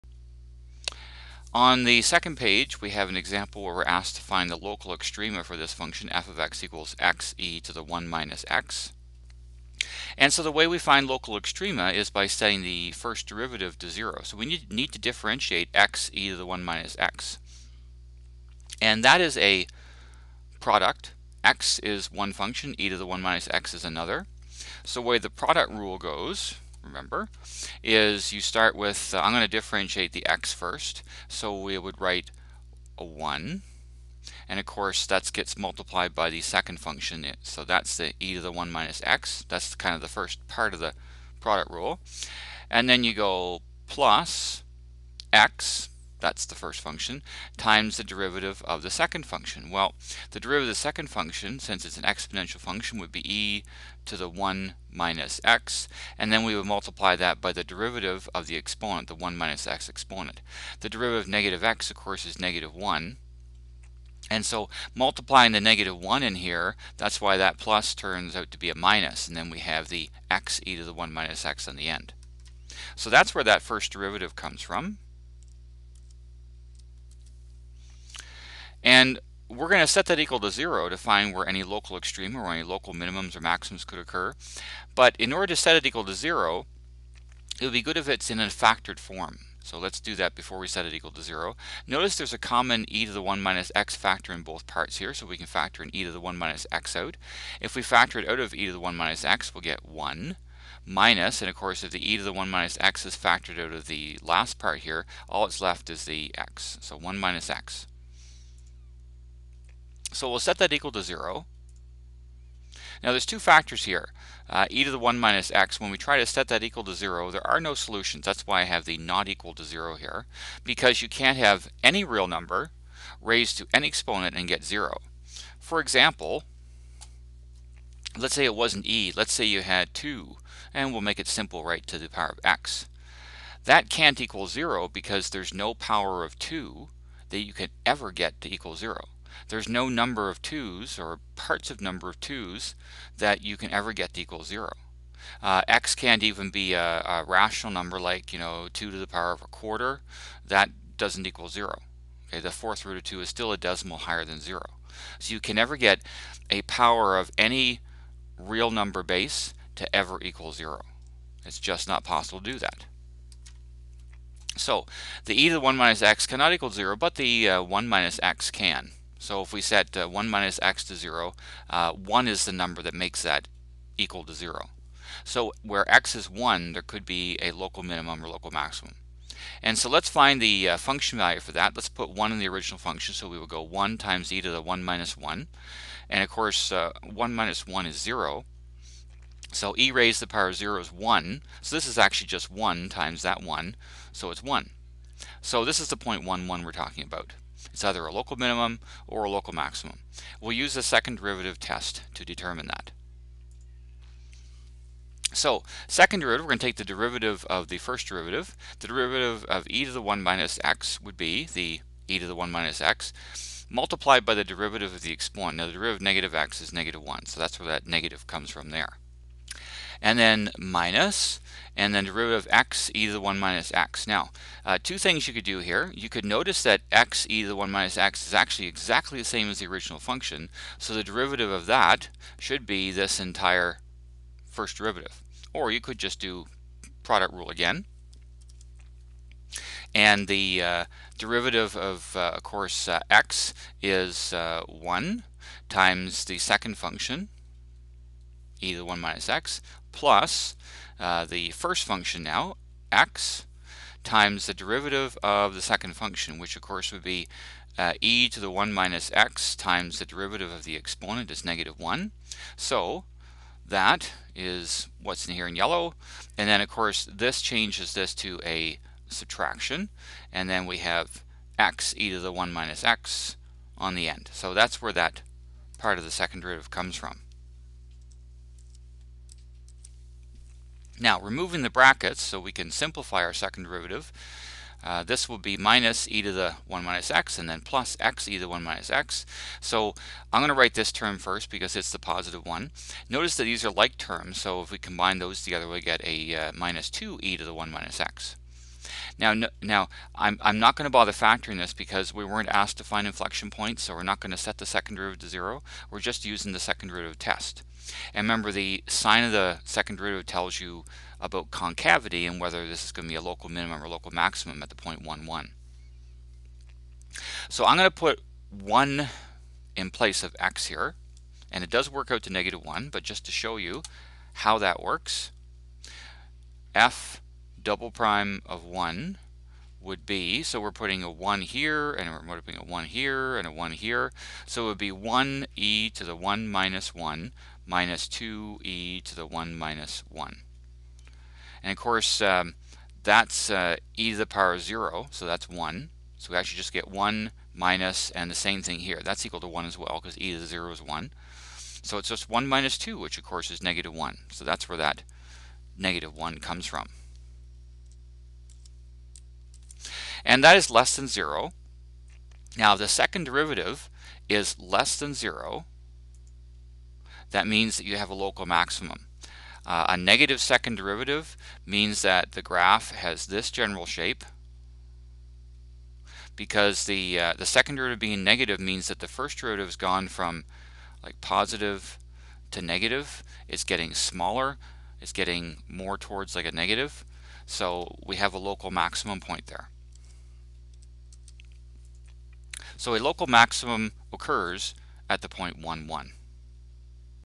Differentiation Rules For Exponential Functions Part 2
This lesson shows how to differentiate functions that involve exponential functions.  The lesson focuses particularly on e^x functions. This is the second part of a two part lesson. This lesson was created for the Calculus and Vectors (MCV4U) course in the province of Ontario, Canada